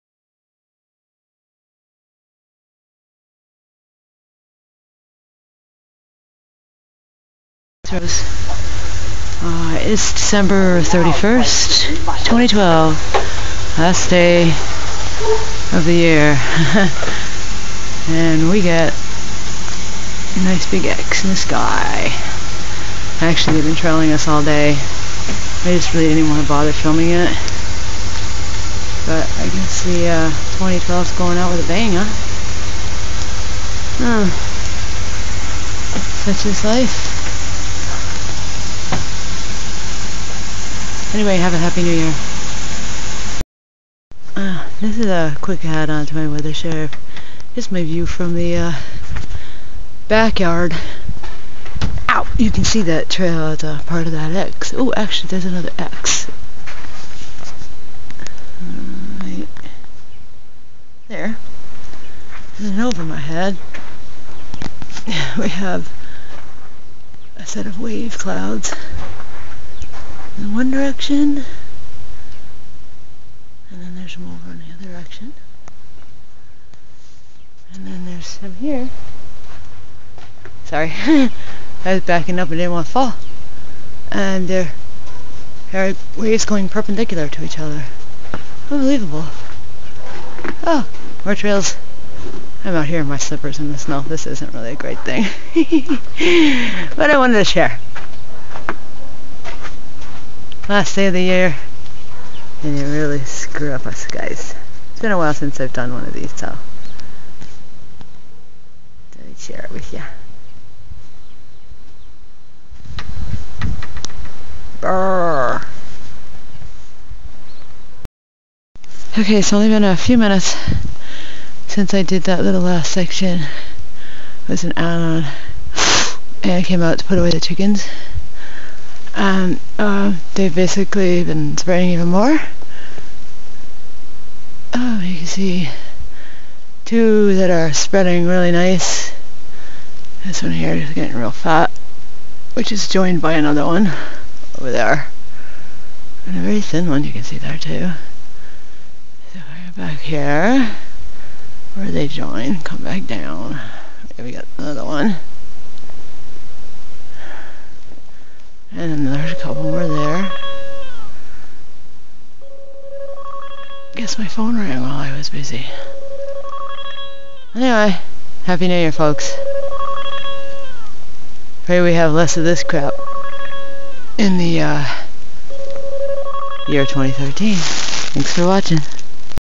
The Last of the trails and waves of 2012
Shot on Dec 31, 2012. They couldn;t let the year go out without messing up the sky here in Central Massachusetts.